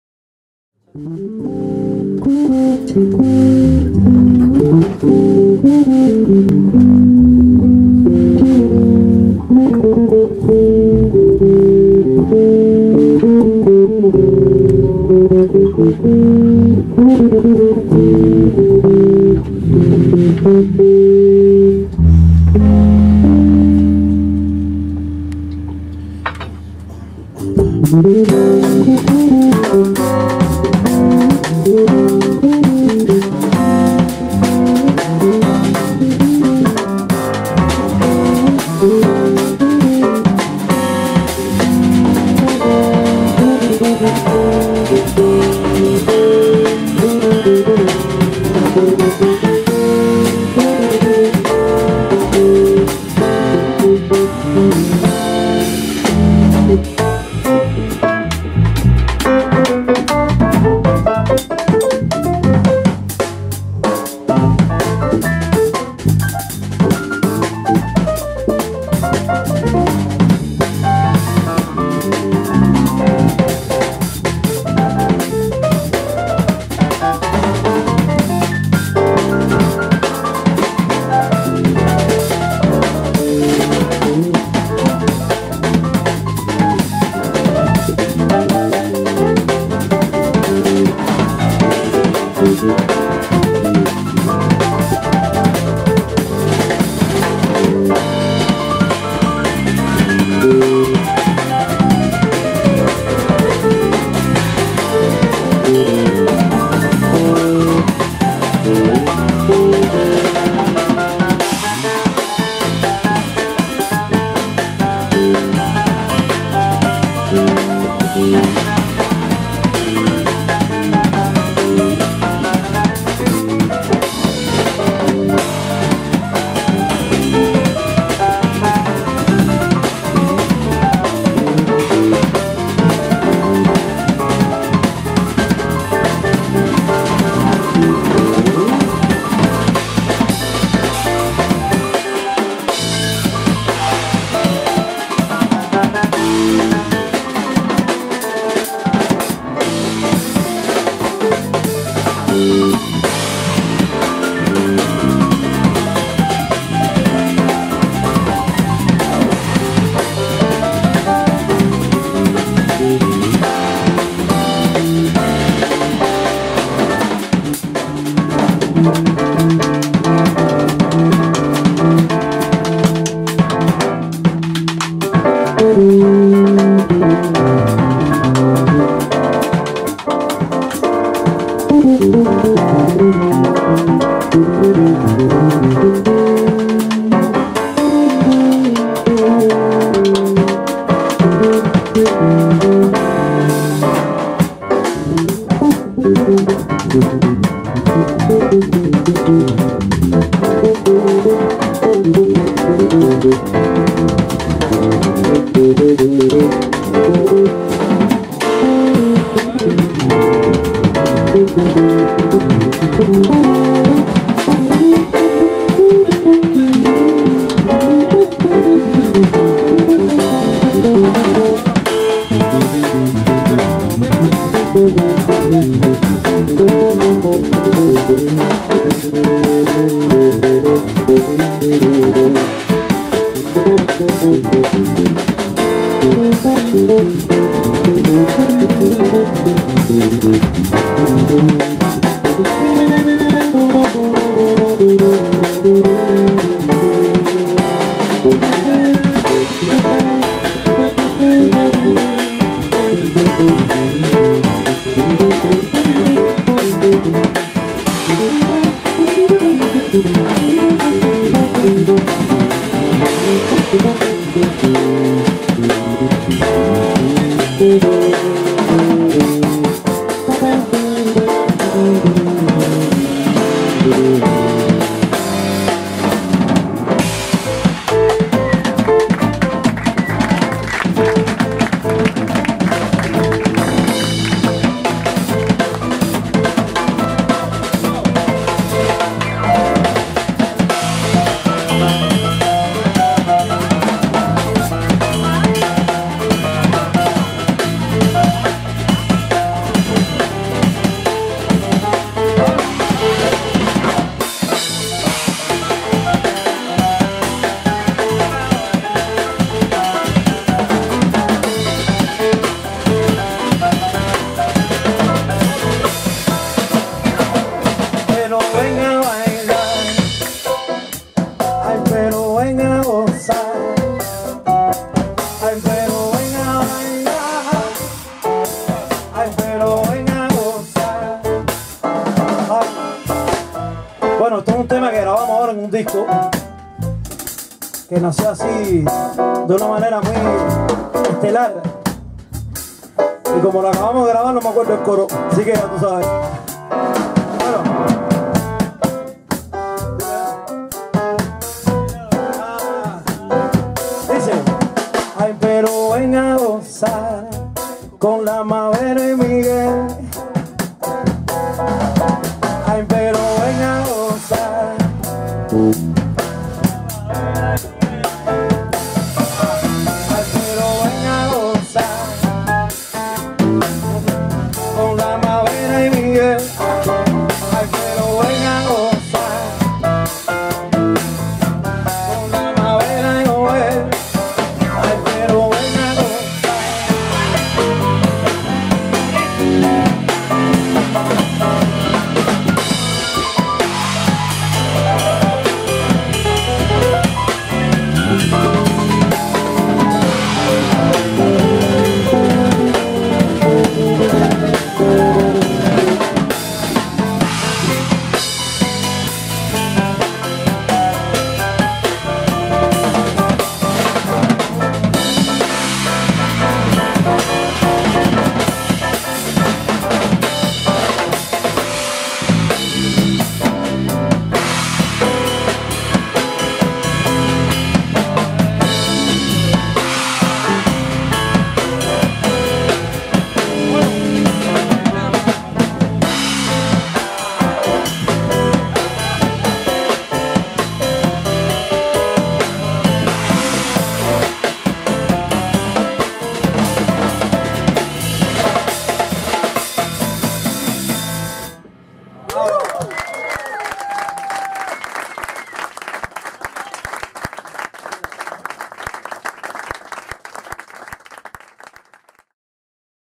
NOEL, VERA & LAMAS (A Coruña, jazz Filloa 8.7.15)
Dany Noel (baixo), Jorge Vera (teclados) e Miguel Lamas (batería).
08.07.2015 | JAZZ FILLOA (A Coruña)